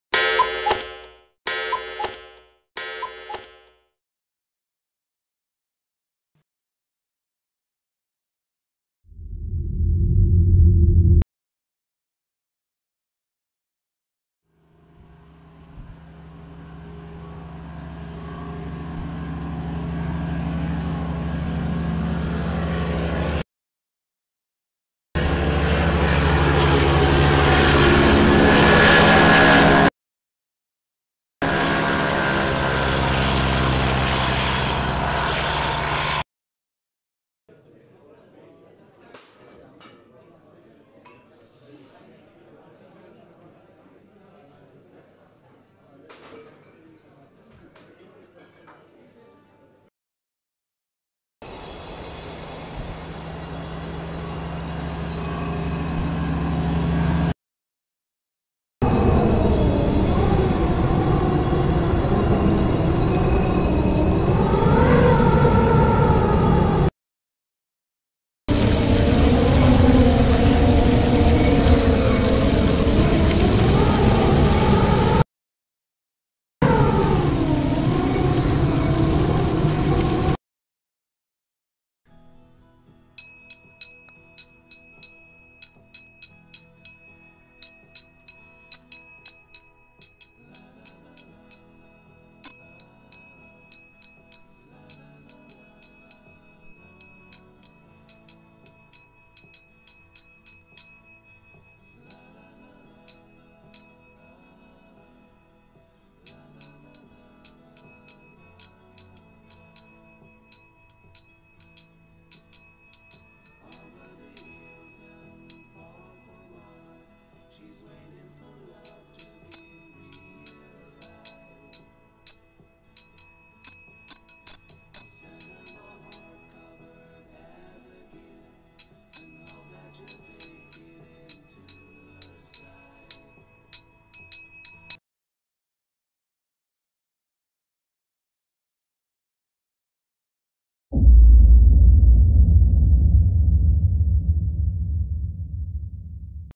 MISS LY criminal cases 4. "The poisoned Santa Claus"
Hilferuf vom Nordpol. Der Weihnachtsmann wurde vergiftet! Miss Ly macht sich auf die beschwerliche Reise zum Nordpol.
Call for help from the North Pole. Santa Claus was poisoned! Miss Ly sets out on the arduous journey to the North Pole.
Pide ayuda desde el Polo Norte. ¡Santa Claus fue envenenado! La señorita Ly emprende el arduo viaje hacia el Polo Norte.